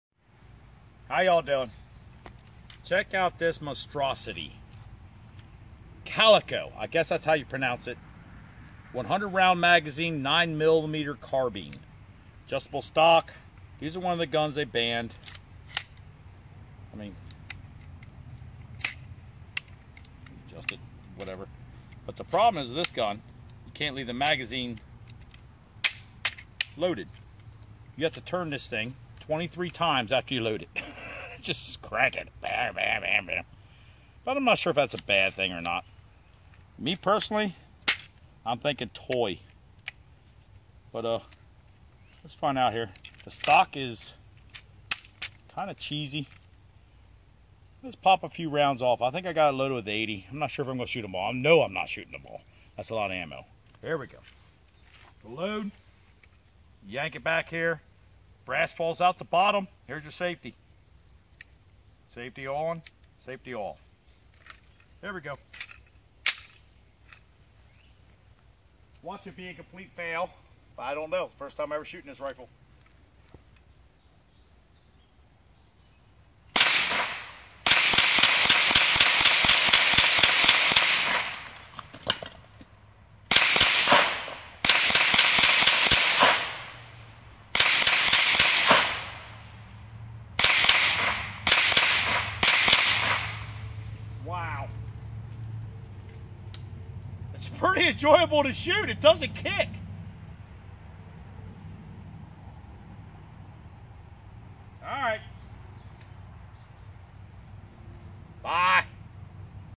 Calico 9 millimeter carbine
I'm shooting this piece of crap although it operated flawless when I hand it to my wife after 10 rounds it acted up.